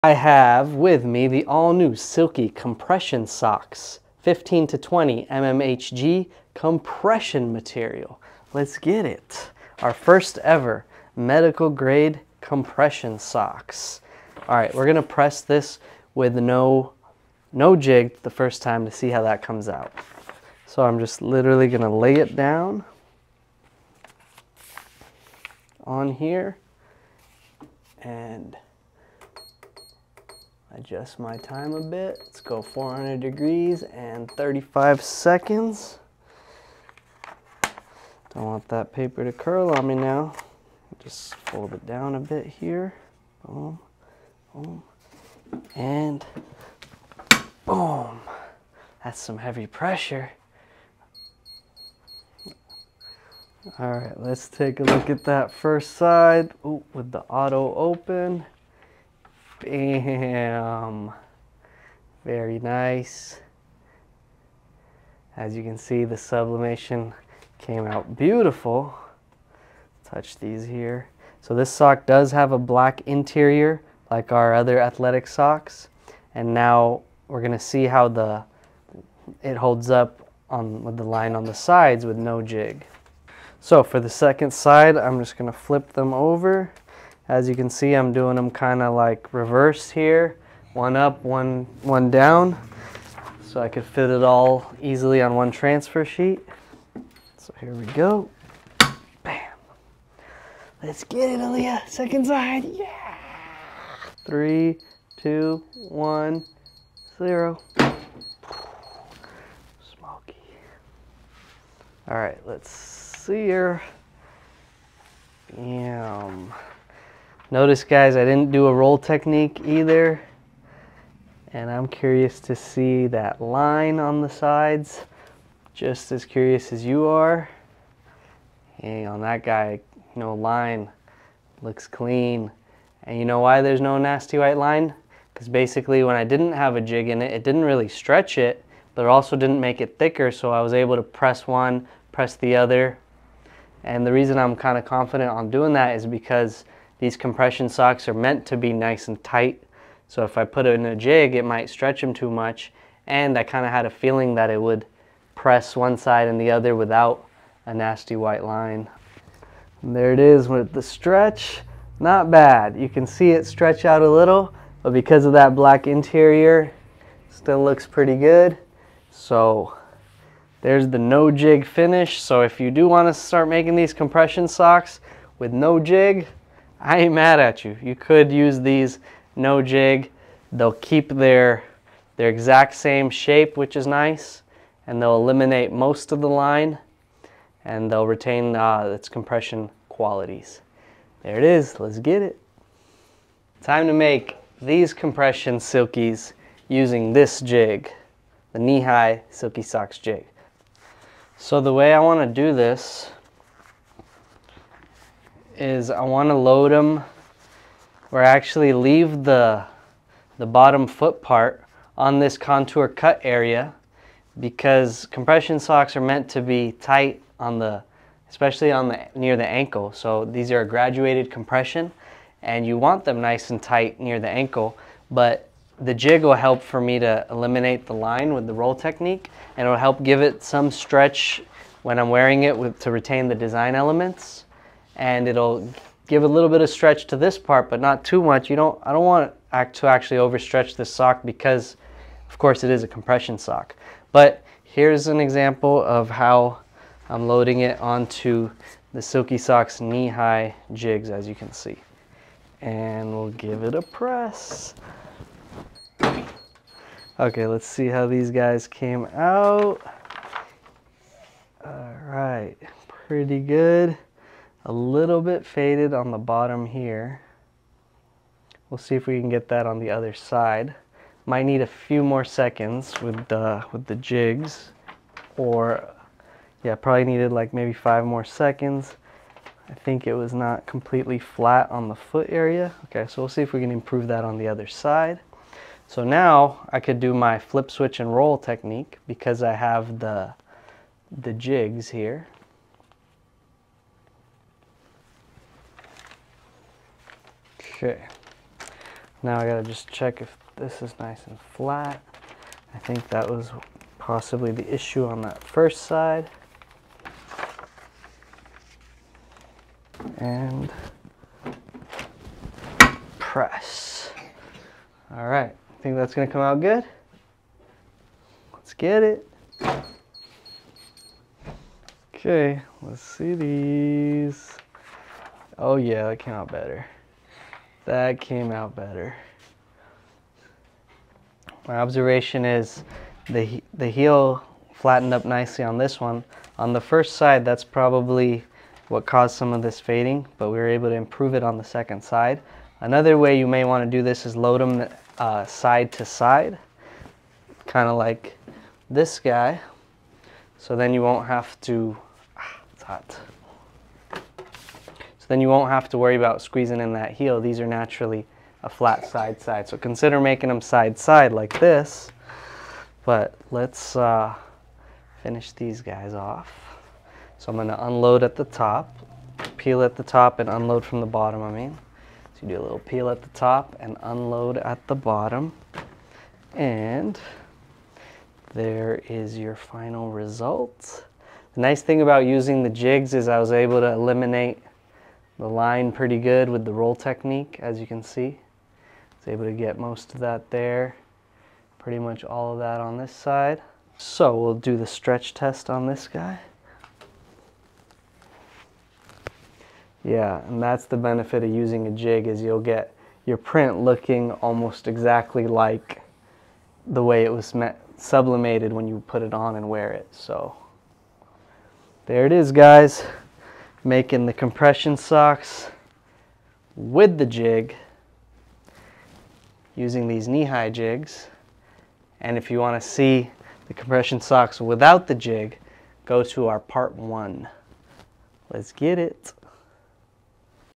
Medical Grade COMPRESSION Socks for Sublimation!
Hey Silky Socks makers and Sublimators!  We have an all new product for you guys, developed for sublimation, and serving a much needed market:  Nurses and people on their feet all day that need to wear compression socks! They can now get these socks customized and sublimated by you guys with your amazing designs. The socks are a mild-medium grade compression with 15-20 mmHg.  And they fit tight enough to be compression socks but also mild enough to allow you to wear them all day with comfort!  They contain the black foot (made from cotton) for comfort on your feet, and a white sublimatable top, with a black interior so it will look great when you wear it!  It can be printed with or without a Jig as this video demonstrates!

Join our FB group where we engage daily and announce our new product releases, sales and what we are working on first

Bless your feet!